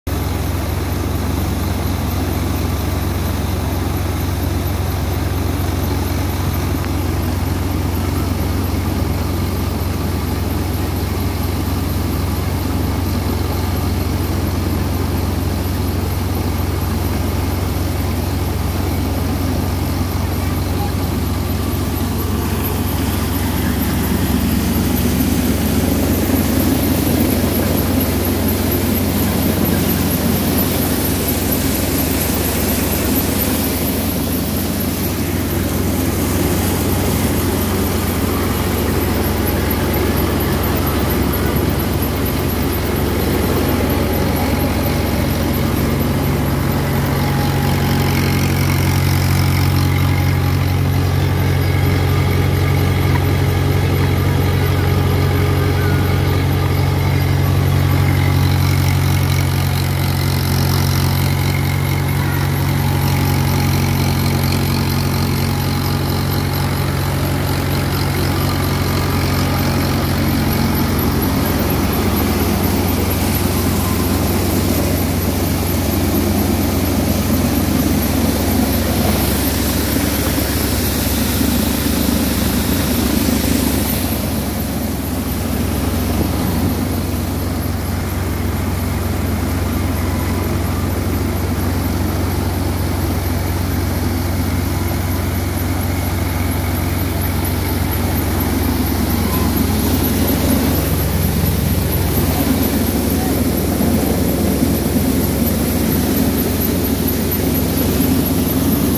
JP ZANDU HARAMBHA WHEAT THRESHER- DIESEL SAVIOUR ON MAHINDRA 575
By Zandu Steel Works, Ambala city-

Size 36x40 with low Diesel consumption technology and load free tractor driven and capacity around 25 quintal per hour.